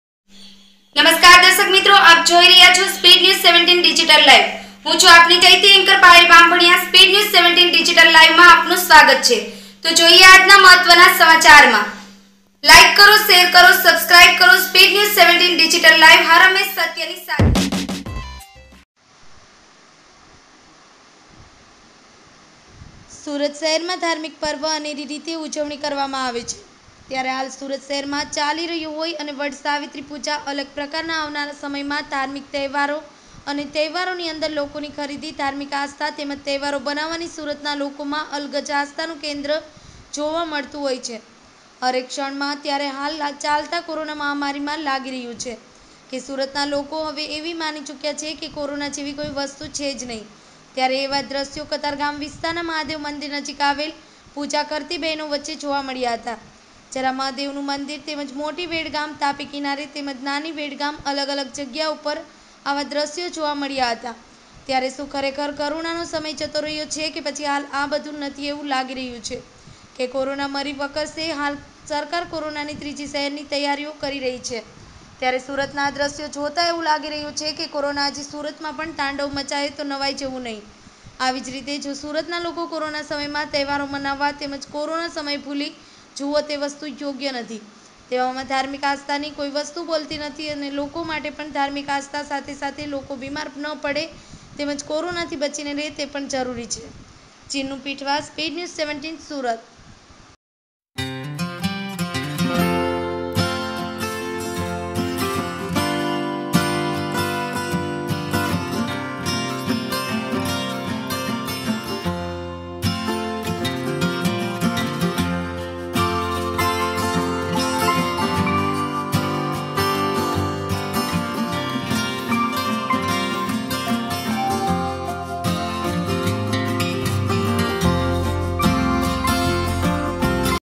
SURAT શહેરમાં ધાર્મિક પર્વ અનેરી રીતે ઉજવણી કરવામાં આવે છે.
સુરત શહેરમાં ધાર્મિક પર્વ અનેરી રીતે ઉજવણી કરવામાં આવે છે.

ત્યારે હાલ સુરત શહેરમાં ચાલી રહ્યો હોય અને વડ સાવિત્રી પૂજા અલગ પ્રકારના આવનારા સમયમાં ધાર્મિક તહેવારો અને તહેવારો ની અંદર લોકોની ખરીદી ધાર્મિક આસ્થા તેમજ તહેવારો બનાવવાની સુરતના લોકોમાં અલગજ આસ્થાનું કેન્દ્ર જોવા મળતું હોય છે હરેક ક્ષણ માં ત્યારે હાલ ચાલતા કોરોના સમયમાં લાગી રહ્યું છે કે સુરતના લોકો હવે એવી માની ચૂક્યા છે કે કોરોના જેવી કોઇ વસ્તુ છે જ ત્યારે એવા જ દ્રશ્યો કતારગામ વિસ્તારના મહાદેવ મંદિર નજીક આવેલ પૂજા કરતી બહેનો વચ્ચે જોવા મળ્યા હતા જરા મહાદેવનું મંદિર તેમજ મોટી વેડ ગામ તાપી કિનારે તેમજ નાની વેડ ગામ અલગ અલગ જગ્યા ઉપર આવા દ્રશ્યો જોવા મળ્યા હતા ત્યારે શું ખરેખર કરુણા નો સમય જતો રહ્યો છે કે પછી હાલ આ બધું નથી એવું લાગી રહ્યું છે કે કોરોના મરી વકરશે હાલ સરકાર કોરોના ની ત્રીજી શહેરની તૈયારીઓ કરી રહી છે ત્યારે સુરત ના આ દ્રશ્ય જોતા એવું લાગી રહ્યું છે કે કોરોના હજી સુરતમાં પણ તાંડવ મચાવે તો નવાઈ જેવું નહીં આવી જ રીતે જો સુરતના લોકો કોરોના સમયમાં તહેવારો મનાવવા તેમજ કોરોના સમય ભૂલી જુઓ તે વસ્તુ યોગ્ય નથી તેવામાં ધાર્મિક આસ્થા થી કોઈ વસ્તુ બોલતી નથી હોતી લોકો માટે પણ ધાર્મિક આસ્થા સાથે સાથે લોક બીમાર ન પડે તેમજ કોરોના થી બચીને રહે તે પણ જરૂરી છે...

રિપોર્ટર ચીનનું પીઠવા સુરત